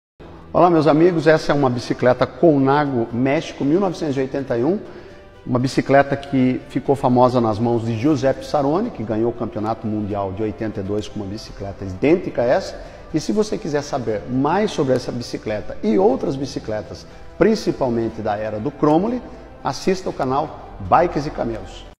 COLNAGO MÉXICO x COLNAGO SARONNI [BIKES DE COMPETIÇÃO]
Você conhece estas duas máquinas ? Estas bikes de competição tem muitas diferenças. Acompanhe

O Canal Bikes e Camelos abordará BICICLETAS E COMPETIÇÕES, com os HERÓIS dessas CORRIDAS e suas MÁQUINAS.